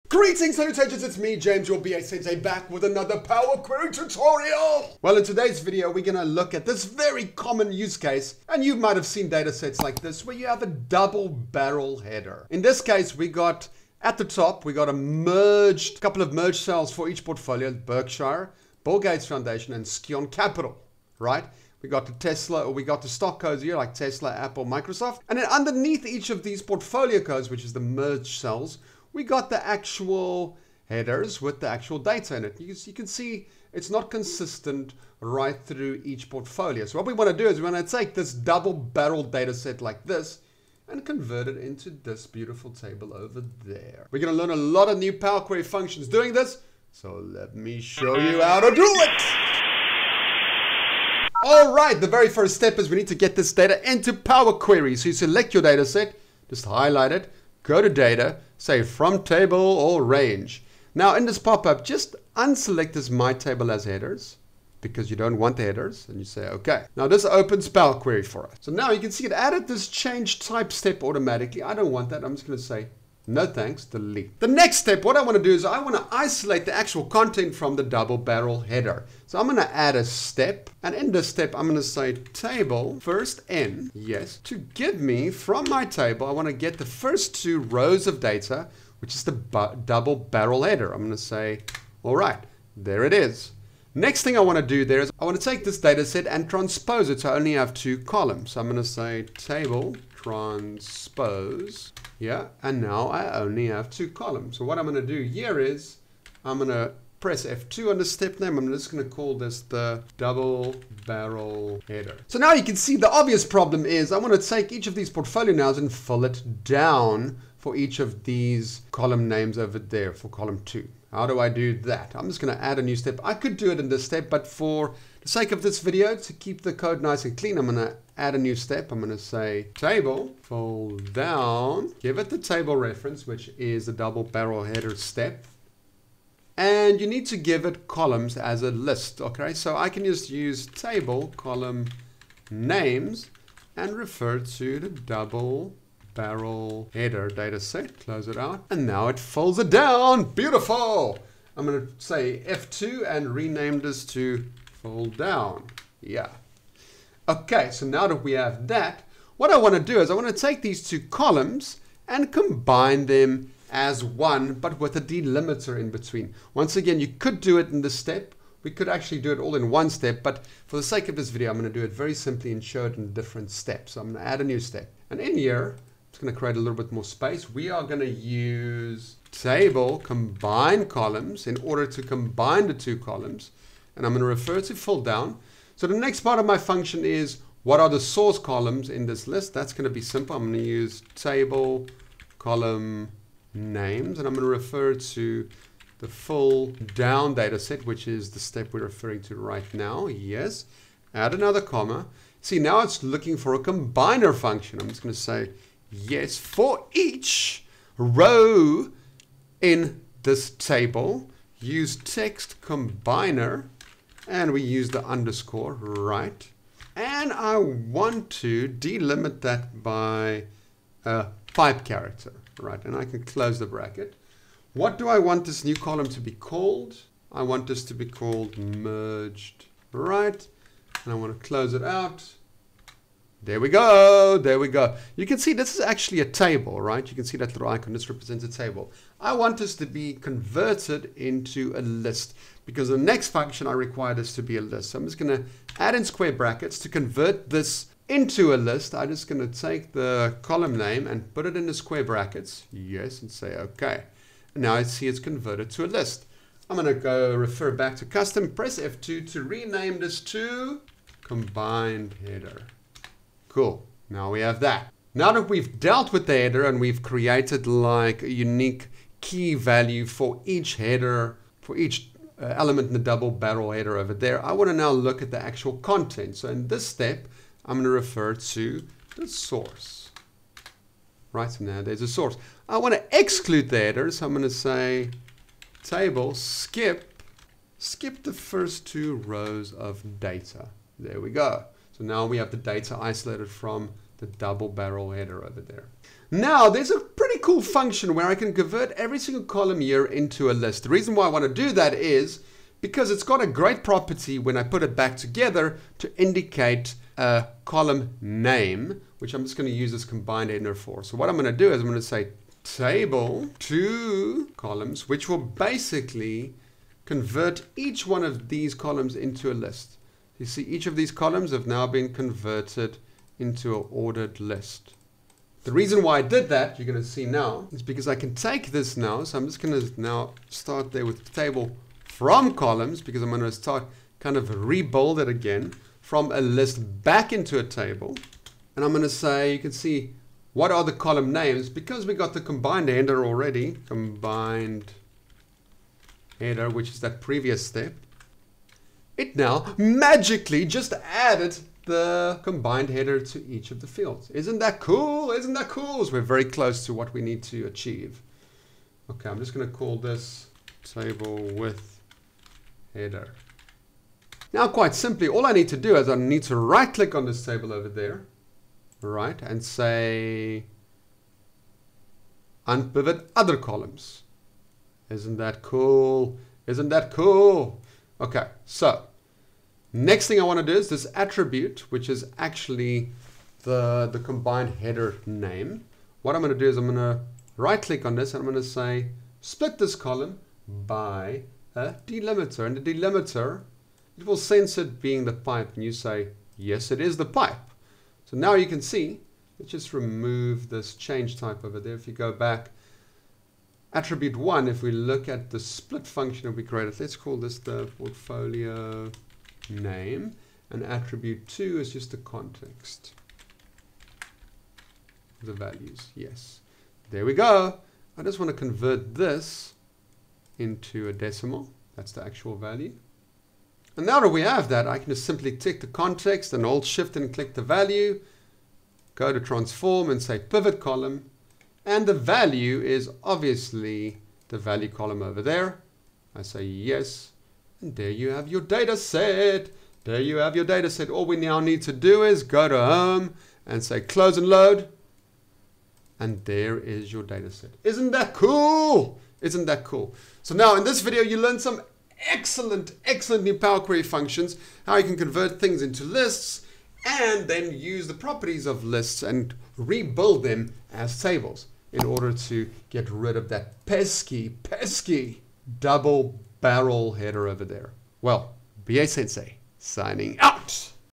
Power Query - Unpivot Double Barrel Headers
In This Video i show you how to deal with the Double Barrel Header problem you'll encounter in your day to day life. You'll get to use some m query functions to unpivot the dreaded double barrel header and clean your data easily.

M Query Functions Used
 • Table.Transpose
 • Table.FirstN
 • Table.CombineColumns
 • Table.ColumnNames
 • Table.ToColumns
 • Table.FromColumns
 • Table.UnpivotOtherColumns
 • Table.SplitColumns